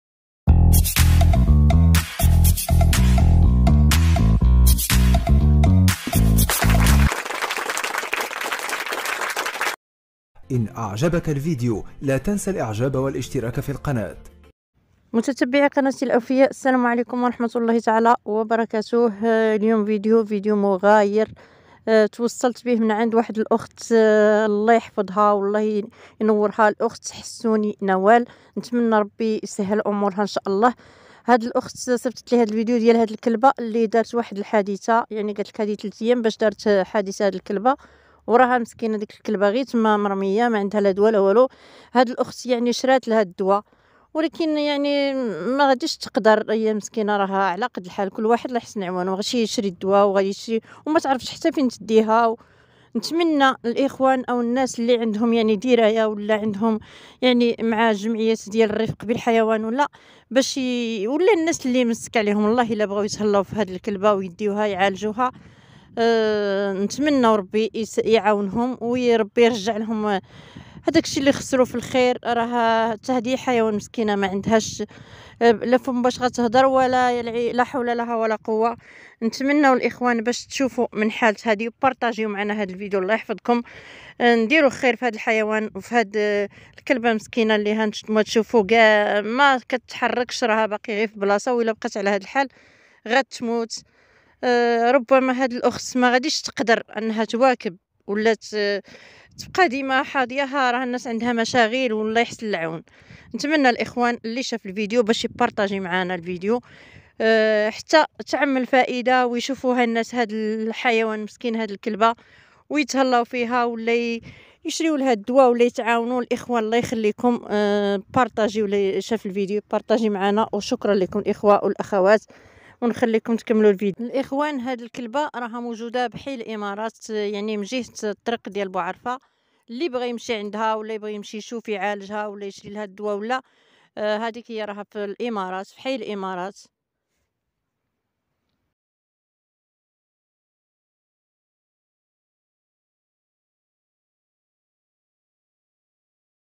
انقذوا هذه الكلبة موجودة بحي الامارات عين بني مطهر
انقذوا هذه الكلية موجودة بحي الامارات عين بني مطهر